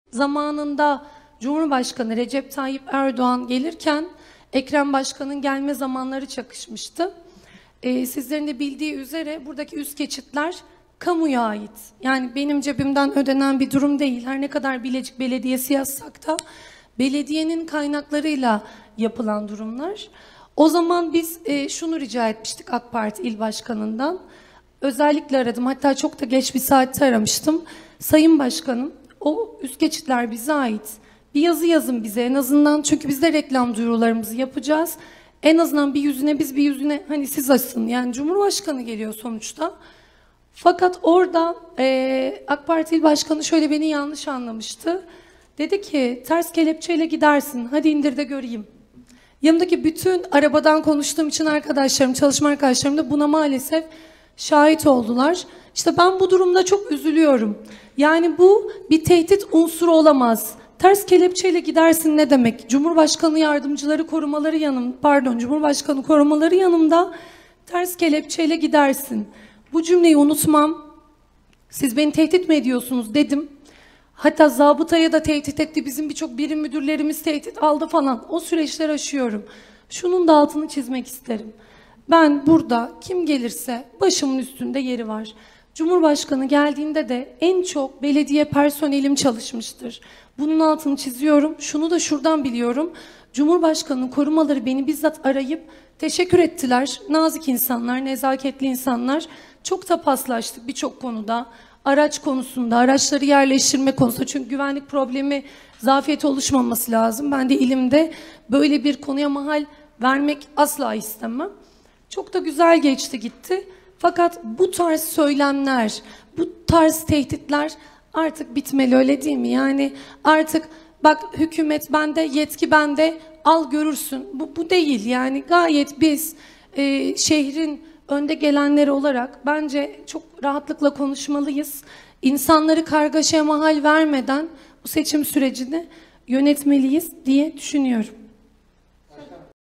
CHP’li belediye başkan vekili Şubaşı 'Ters kelepçe ile gidersin’ diye tehdit edildim
Bilecik Belediye Başkan Vekili Melek Mızrak Subaşı, Bilecik Belediyesi 2023 Yılı Mayıs Ayı Meclis Toplantısı 1. Birleşiminde çarpıcı açıklamalarda bulundu.

Bilecik Belediye Başkan Vekili Melek Mızrak Subaşı, meclis toplantısında gündem maddeleri görüşüldükten sonra meclis üyelerinin sorularını yanıtlarken, merkezde bulunan parklarda kurulan siyasi partilerin çadır ve stant kurulumları tartışma konusu olurken Başkan Vekili Subaşı AK Parti İl Başkanı Serkan Yıldırım'ın kendisini tehdit ettiğini öne sürdü.